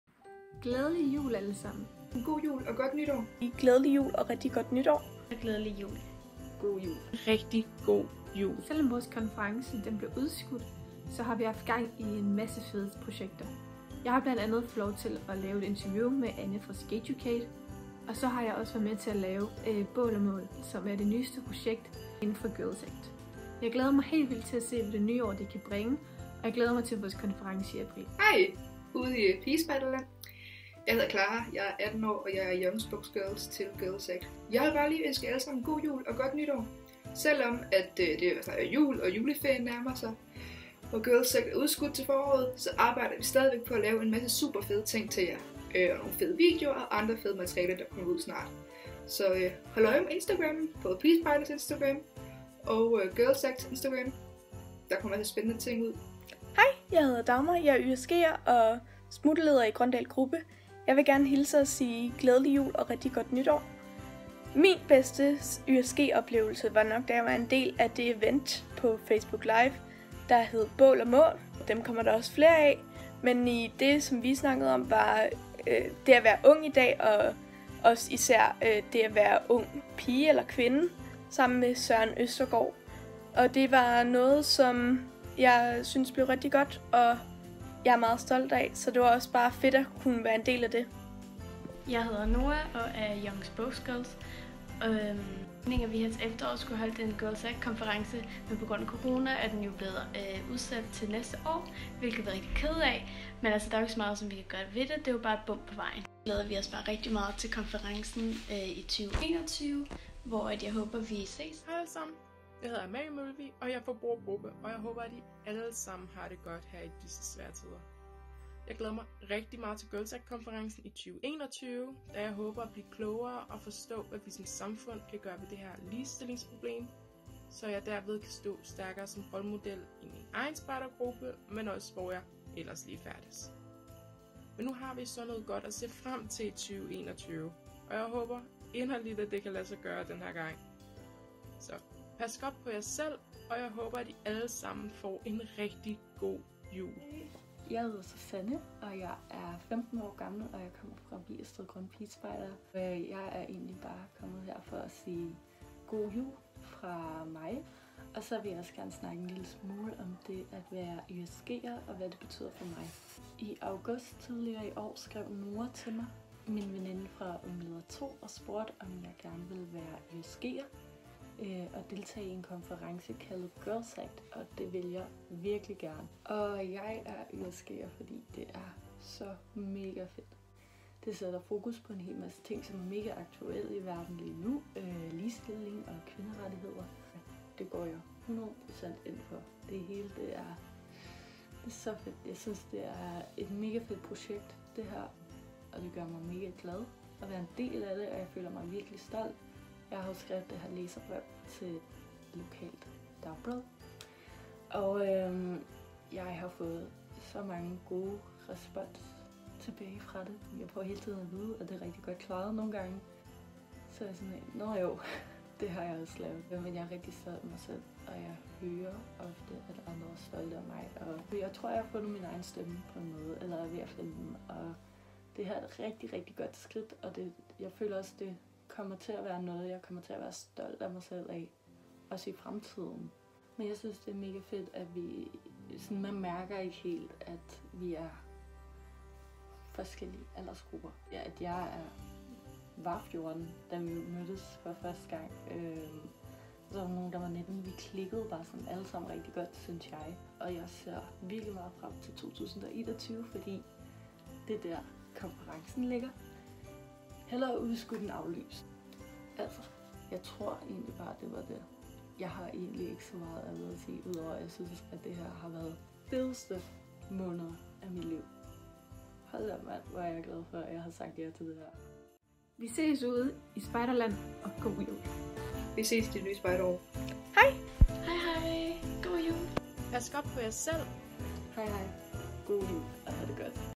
Young Spokes Girls, GIRLS ACT, julehilsen
De grønne pigespejderes Young Spokes Girls, sender en julehilsen til alle medlemmer. Hør dem fortælle om, hvad de allerede har lært som Young Spokes Girls og hvorfor de synes det er for fedt at være med i GIRLS ACT-bevægelsen.